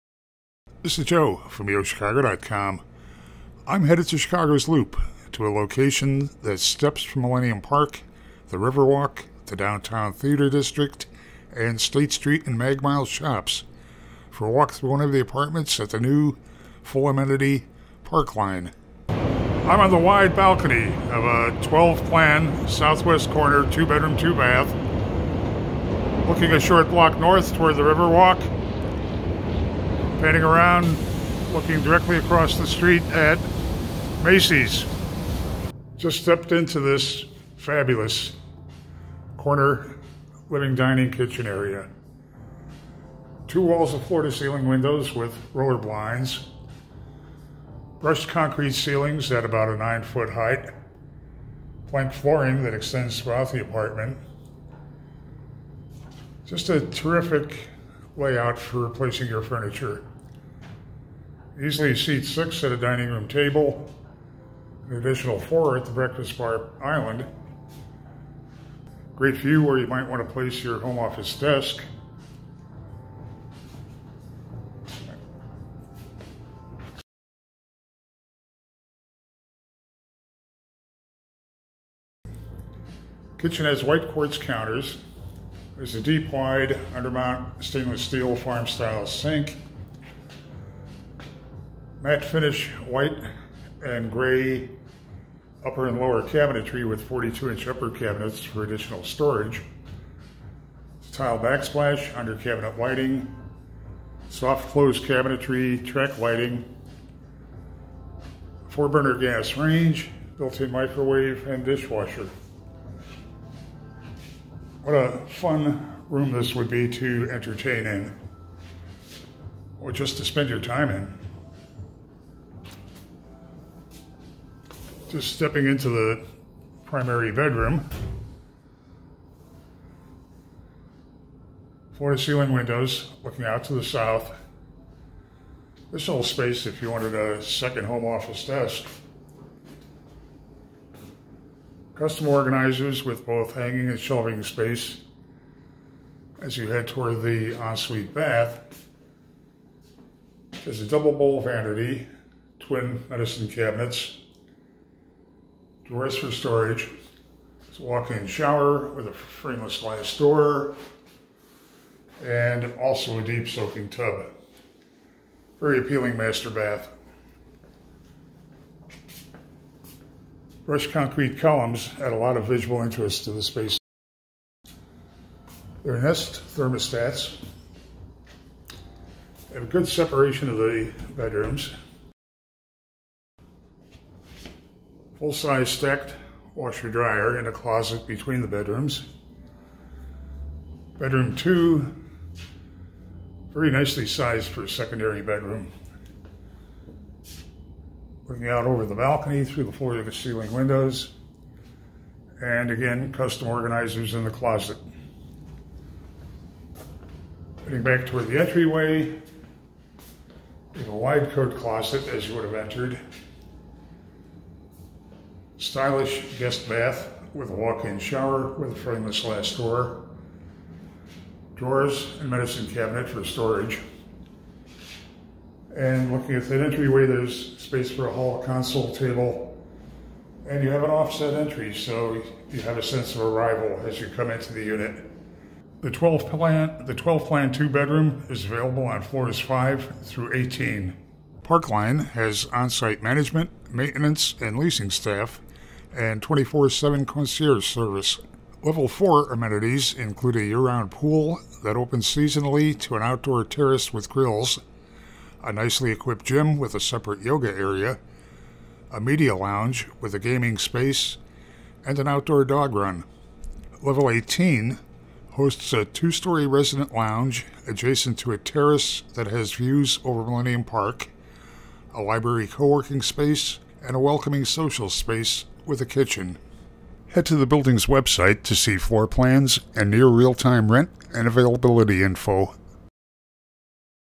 A -12 2-bedroom, 2-bath model at the Loop's new Parkline Chicago
Parkline is a new, full-amenity apartment tower steps from Millennium Park, the Riverwalk, the downtown Theater District, and State Street and Mag Mile shops.

Parkline has studio to 3-bedroom apartments with very livable layouts that include a home office space. All of the apartments have high ceilings, floor-to-ceiling windows with roller blinds, plank flooring throughout, in-unit washer / dryers and upscale kitchen and bath finishes.

Level 4 amenities include a year-round pool that opens seasonally to an outdoor terrace with grills, a nicely-equipped gym with a separate yoga area, a media lounge with a gaming space and an outdoor dog run.

Level 18 hosts a 2-story resident lounge adjacent to a terrace with views over Millennium Park, a library co-working space, and a welcoming social space with a kitchen.

Parkline has on-site management, maintenance and leasing staff and 24/7 concierge service.